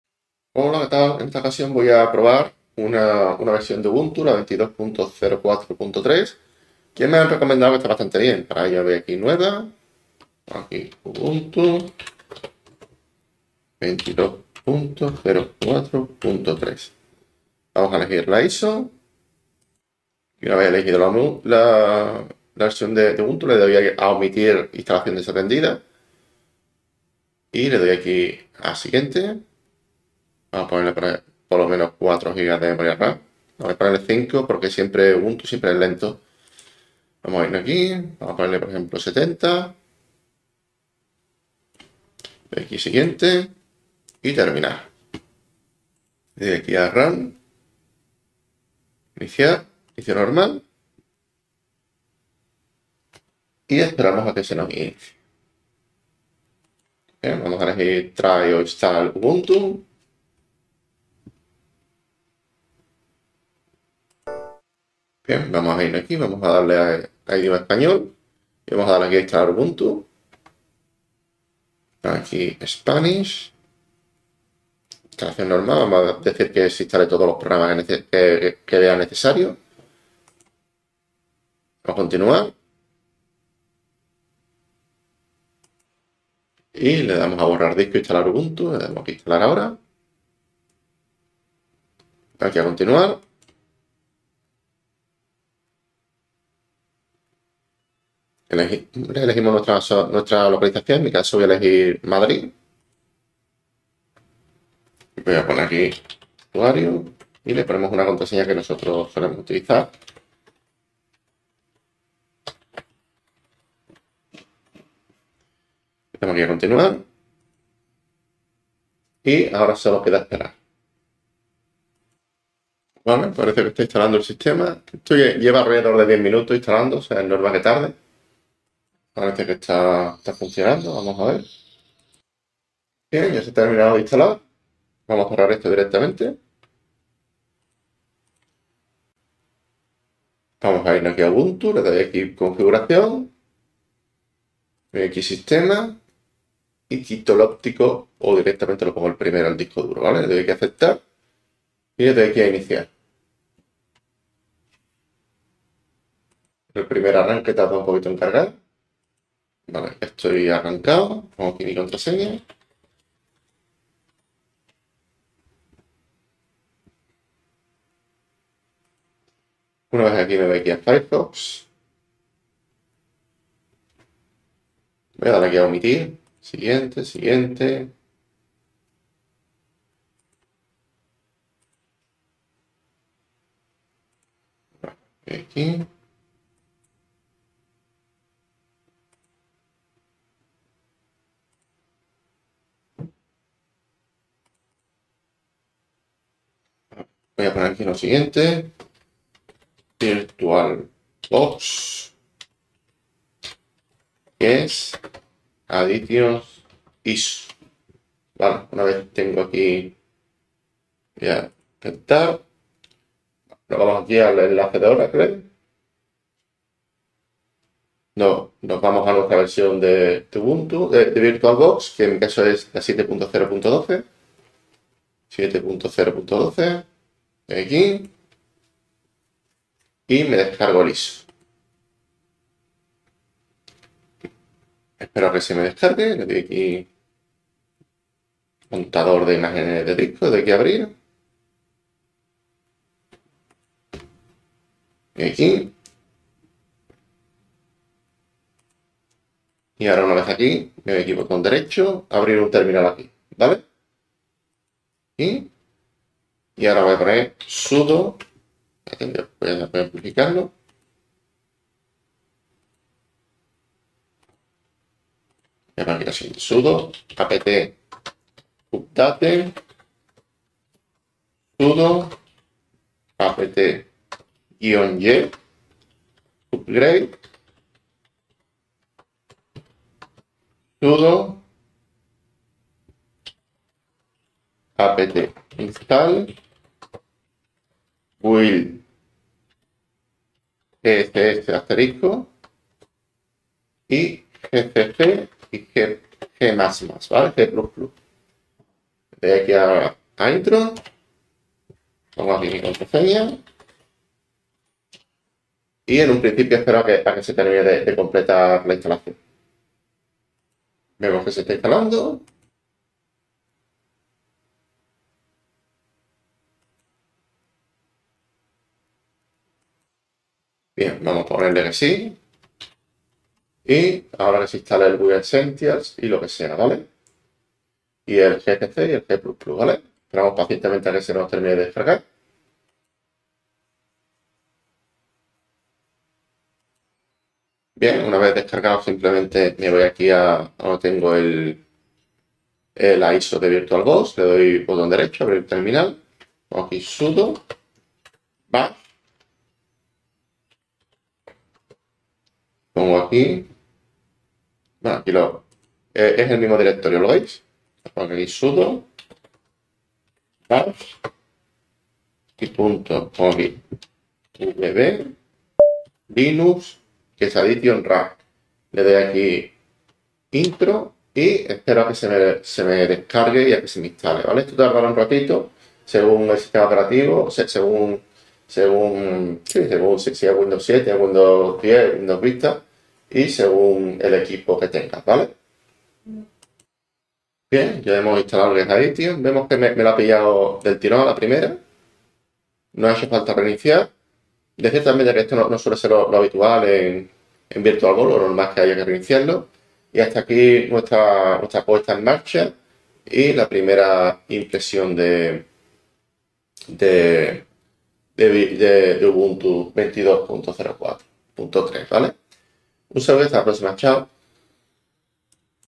Instalación, review y primera impresión de Ubuntu 22.04.3 en VirtualBox
Por aquí dejo cómo instalar Ubuntu 22.04.3 y la instalación del Guest Additions.